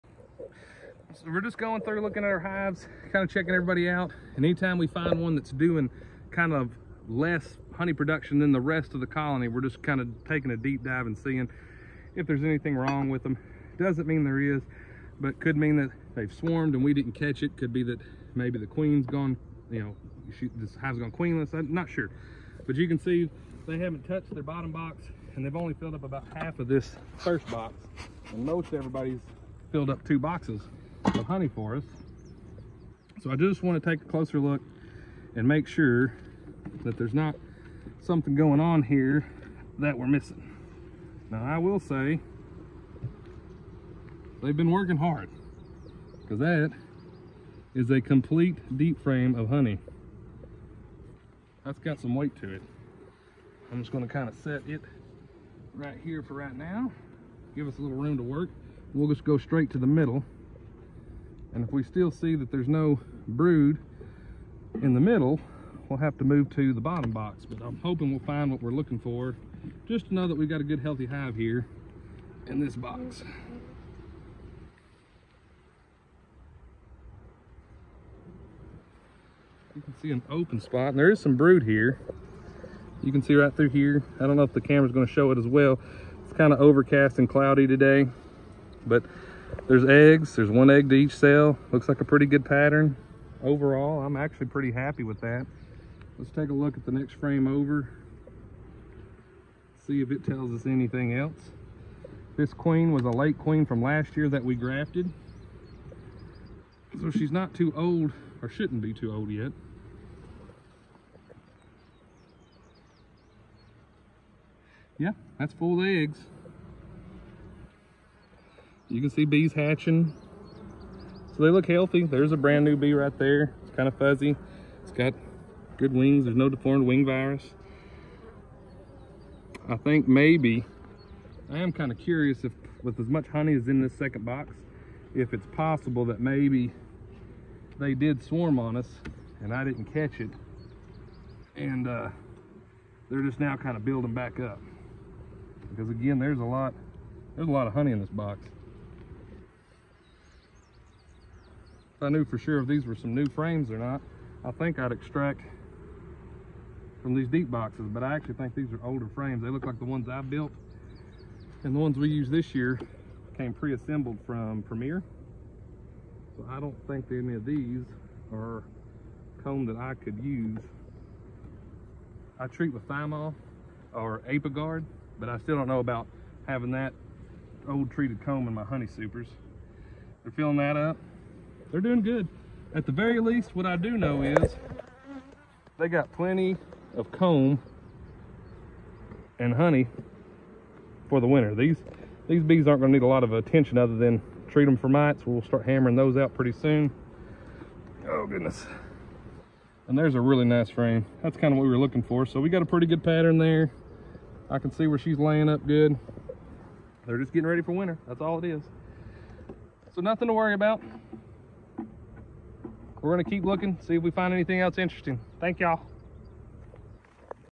Deep Dive on an Unproductive Hive
Checking to see what is going on in our hive that didn’t produce any honey in the supers.